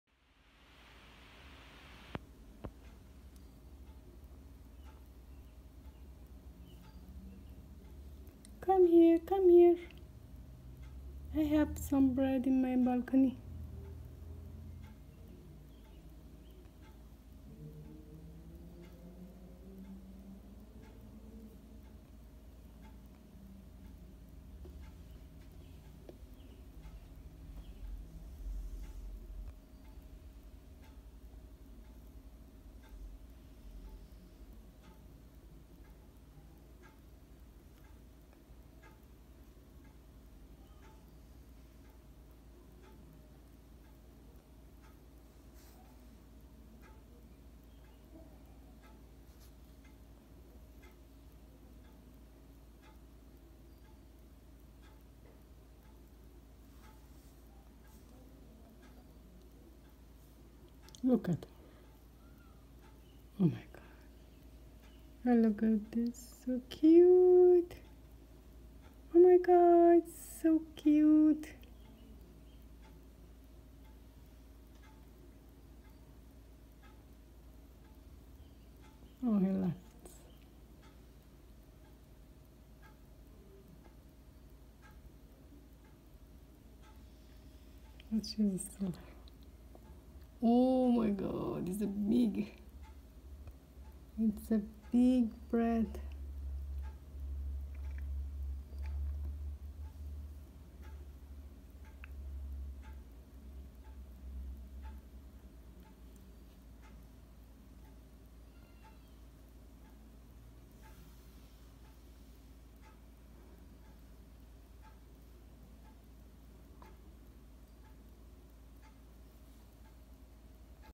Squirrel came to ask where she got food, she didn't car and squirrel left
squirrel didn't care about her friend and she left